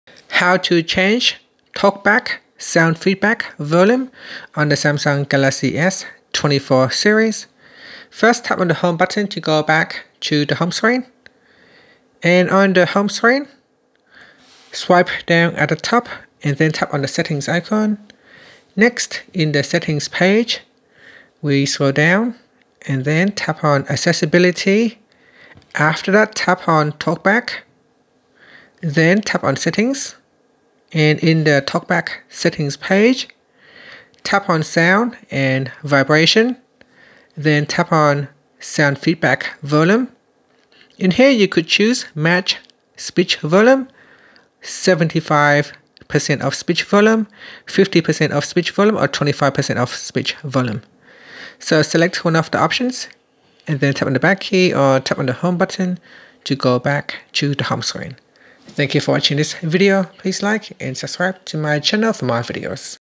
🔊 Galaxy S24/S24+ Ultra: How to Adjust Talkback Volume for Crystal Clear Audio Feedback
In this video, I will show you how to change Talkback sound feedback volume on the Galaxy S24/S24+/S24 Ultra. Is your Galaxy S24's Talkback volume too loud or too quiet? I'll show you how to easily adjust the sound feedback volume for Talkback.